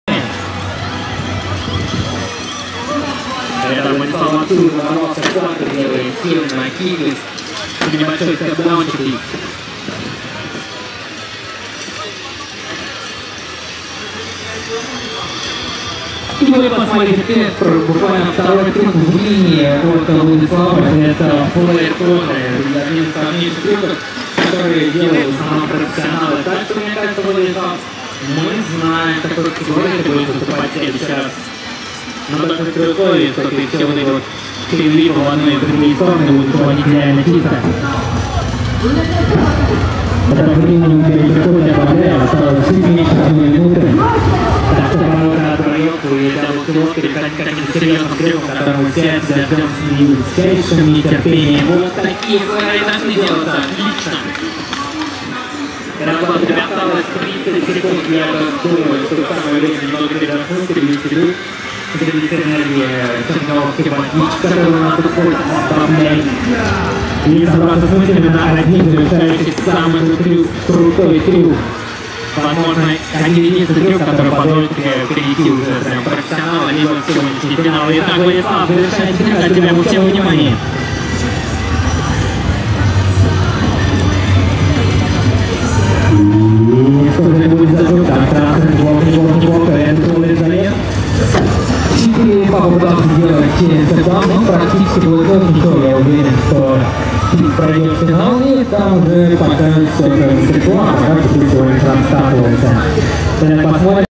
Moscow SCOOTER Games - Арефьев Владислав, любители 20170729
29 июля 2017 в Лужниках прошел контест по экстремальному самокату Moscow SCOOTER Games

 Root Industries и Blunt Scooters, защитная экипировка CHILDREN OF STREET,